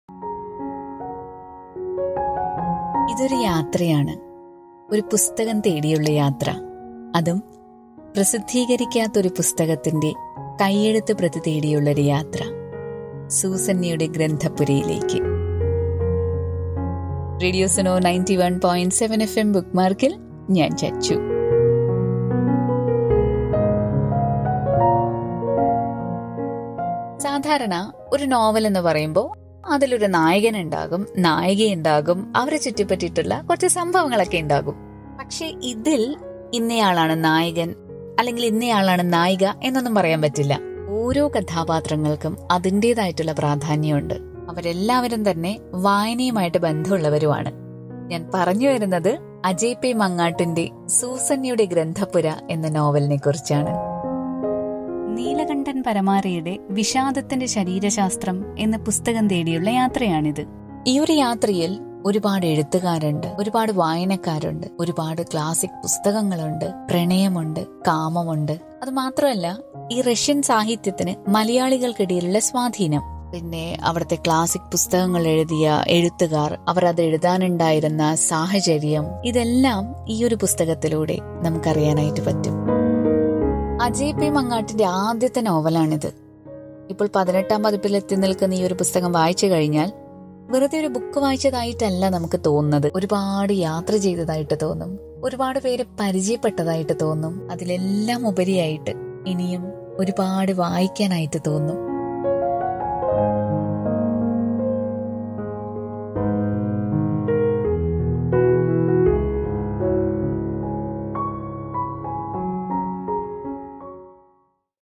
ബെസ്ററ് സെല്ലെർ പട്ടികയിൽ ഇടം നേടിയ അജയ് പി മങ്ങാട്ടിന്റെ നോവൽ സൂസന്നയുടെ ഗ്രന്ഥപ്പുര | RADIO SUNO
ബെസ്ററ് സെല്ലെർ പട്ടികയിൽ ഇടം നേടിയ അജയ് പി മങ്ങാട്ടിന്റെ ആദ്യ നോവൽ
സൂസന്നയുടെ ഗ്രന്ഥപ്പുര
Ajai P Mangattu